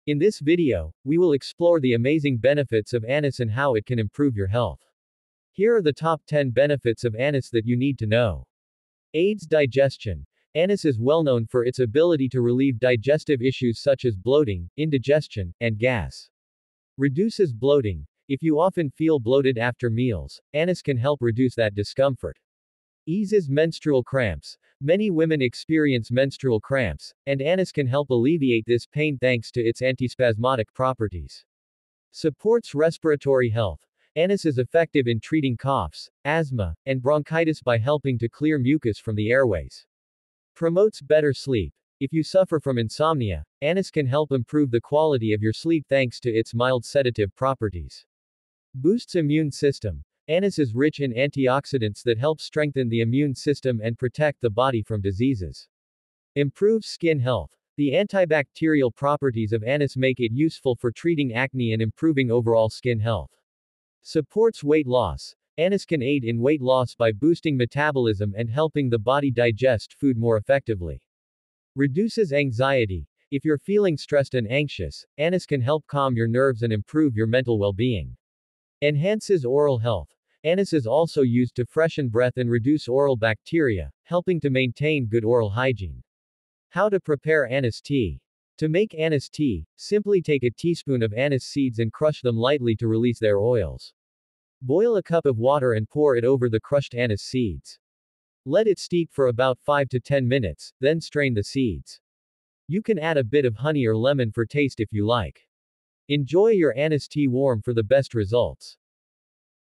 Remedy Natural | How to Use Anise Seeds for Better Health
Discover the amazing health benefits of anise (also known as aniseed) in this comprehensive guide. Anise is a powerful herb known for its distinctive flavor and numerous health advantages. In this video, we’ll dive into the top 10 benefits of anise, including how it can aid digestion, reduce bloating, and support weight loss. Whether you're interested in natural remedies or simply looking to improve your overall well-being, anise seeds are a must-have in your herbal collection
You'll learn how to prepare anise tea to relieve digestive issues, how to incorporate anise into your daily routine for better health, and the best uses of anise in natural remedies. Watch until the end to discover some surprising ways anise can enhance your life
Why You Should Watch:
Learn how anise can improve digestion and reduce bloating.
Discover how to use anise seeds for weight loss and other health benefits.
Get practical tips on incorporating anise into your daily health regimen